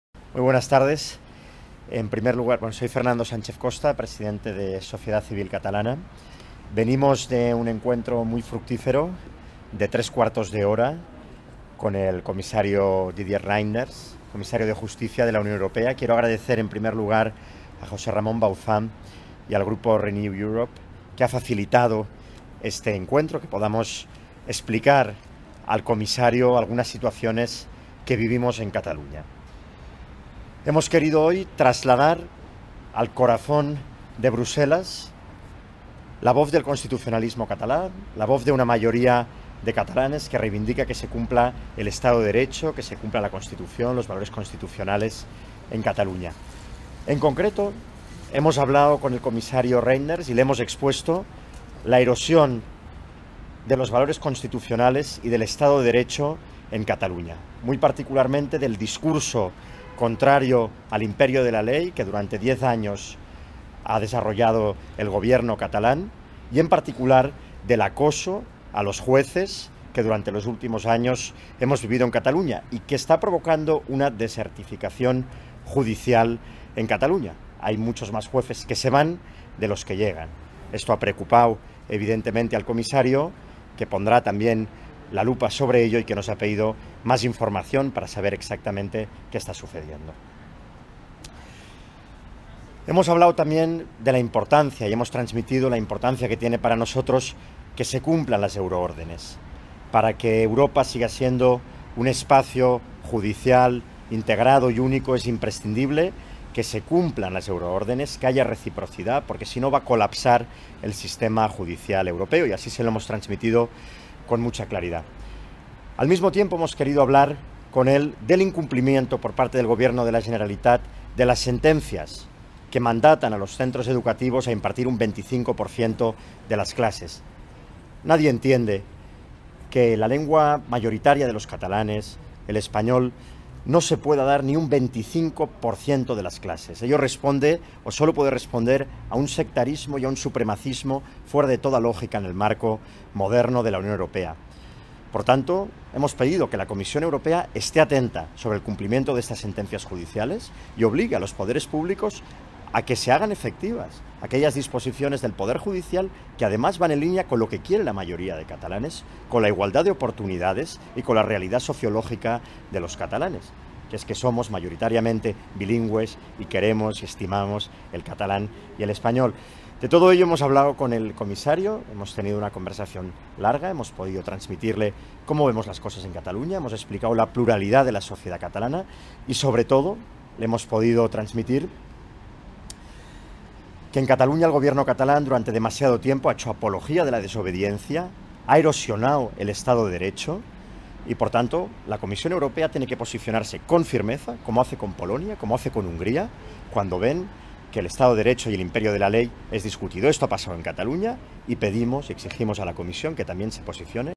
Declaraciones de Sánchez Costa en Bruselas tras entrevistarse con el comisario Didier Reynders.
👉 SOCIETAT CIVIL CATALANA